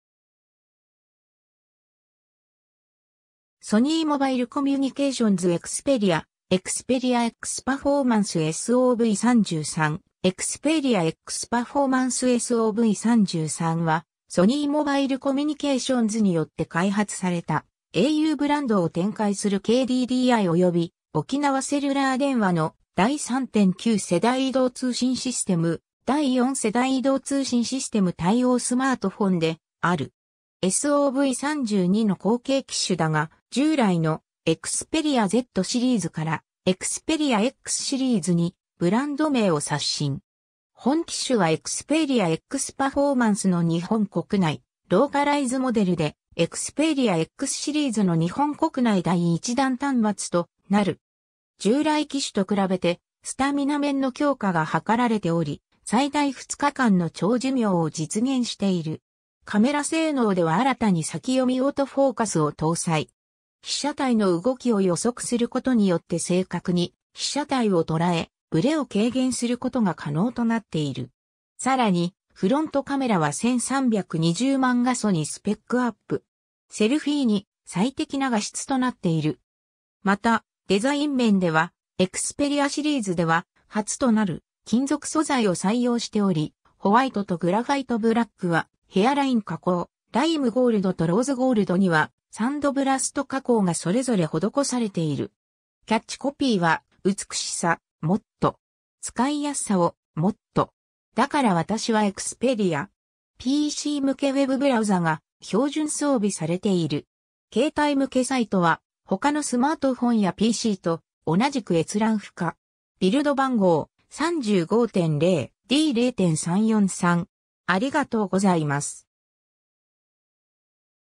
SOV33
携帯電話端末_(au_第四世代)
WiMAX_2.1端末_(au)
携帯電話端末_(au_第3.9世代)
携帯電話端末_(ソニーモバイルコミュニケーションズ)
Android端末_(ソニー)
ソニーモバイルコミュニケーションズ 」 Xperia 」 Xperia X Performance 」 SOV33 Xperia X Performance SOV33（エクスペリア エックス パフォーマンス エスオーブイ サンサン）は、ソニーモバイルコミュニケーションズによって開発された、auブランドを展開するKDDIおよび沖縄セルラー電話の第3.9世代移動通信システム（au 4G LTE/au VoLTE）、第4世代移動通信システム（au 4G LTE CA/WiMAX2+）対応スマートフォンである。
 SOV32の後継機種だが、従来の「Xperia Z」シリーズから「Xperia X」シリーズにブランド名を刷新。
本機種はXperia X Performanceの日本国内ローカライズモデルで、Xperia Xシリーズの日本国内第1弾端末となる。
 従来機種と比べてスタミナ面の強化が図られており、最大2日間の長寿命を実現している。
 カメラ性能では新たに先読みオートフォーカスを搭載。
被写体の動きを予測することによって正確に被写体をとらえ、ブレを軽減する事が可能となっている。
さらに、フロントカメラは1320万画素にスペックアップ。
セルフィーに最適な画質となっている。
 また、デザイン面ではXperiaシリーズでは初となる金属素材を採用しており、WhiteとGraphite Blackはヘアライン加工、Lime GoldとRose Goldにはサンドブラスト加工がそれぞれ施されている。
 キャッチコピーは「美しさ、もっと。使いやすさを、もっと。だから私はXperia。」。
 ※PC向けWebブラウザが標準装備されている。
携帯向けサイト(EZWeb)は他のスマートフォンやPCと同じく閲覧不可。
 ビルド番号：35.0.D.0.343